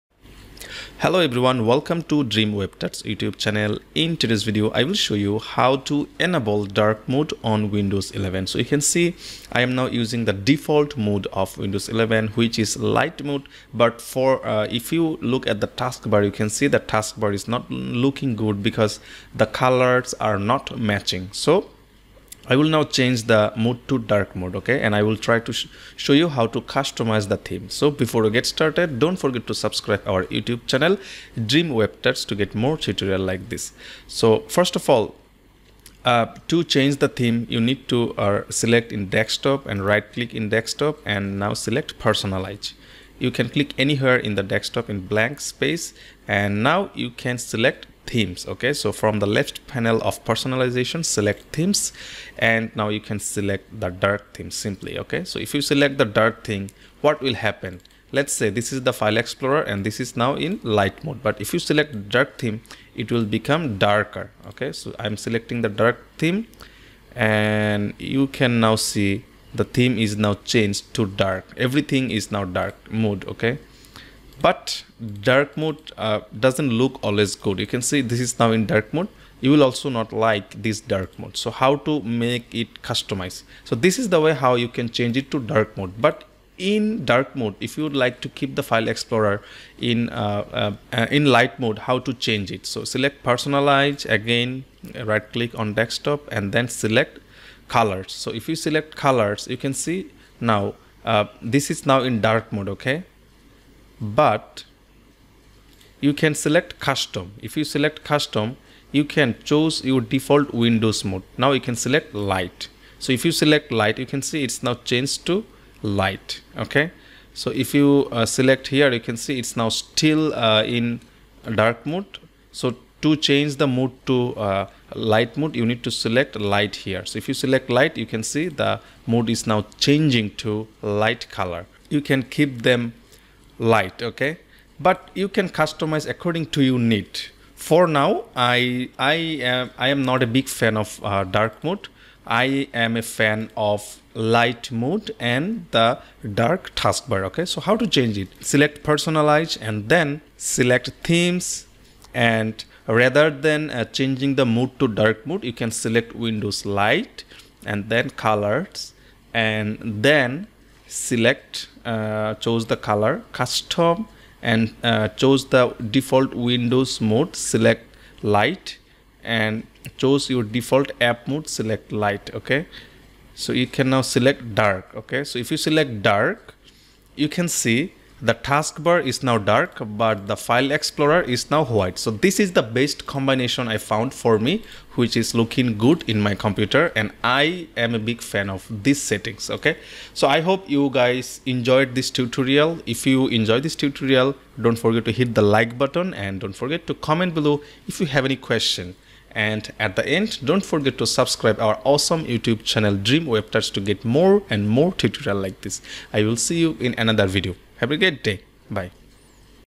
How to Enable Dark Mode on Windows 11
You can use Dark Mode in Windows 11 which is a great feature of Windows 11. You can use this feature to enjoy the dark mode in Windows 11. I have explained this in details in this video how to enable dark mode in Windows 11 and how to customize the taskbar to make it more  beautiful same as me.